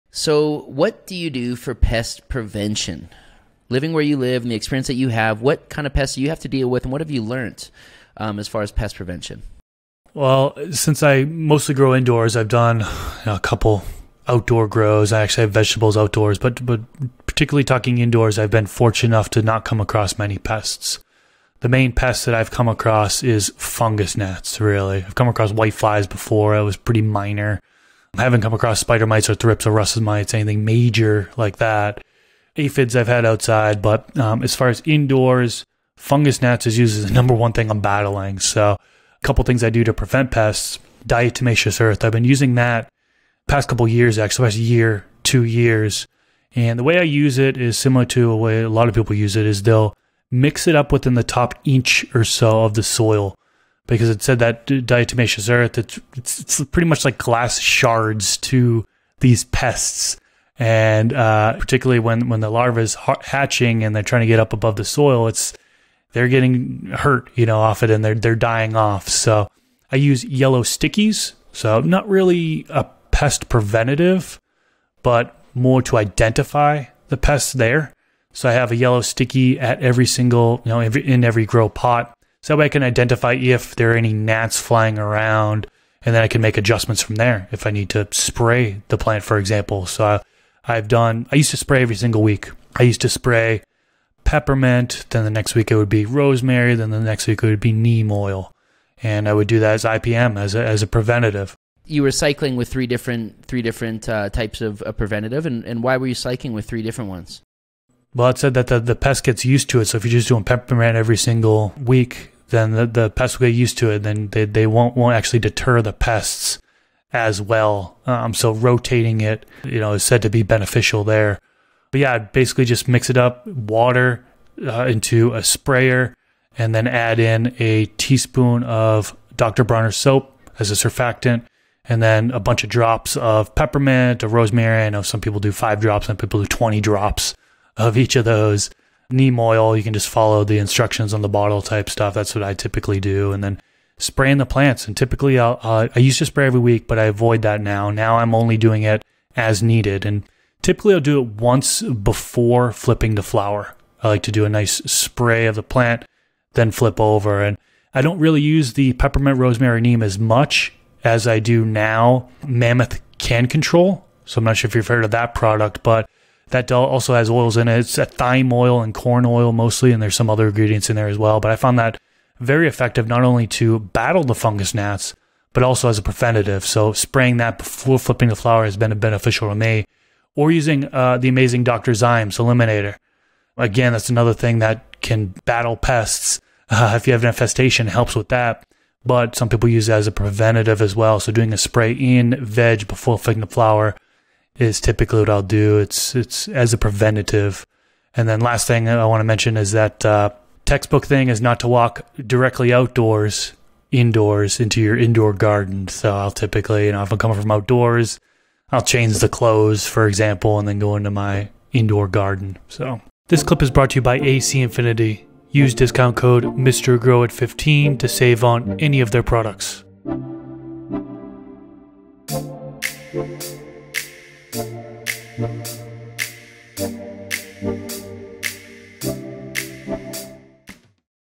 Easy Methods For Pest Prevention In Your Garden!
In this Garden Talk clip, Mr. Grow It talks about some easy methods for pest prevention in your garden. What are your thoughts on this?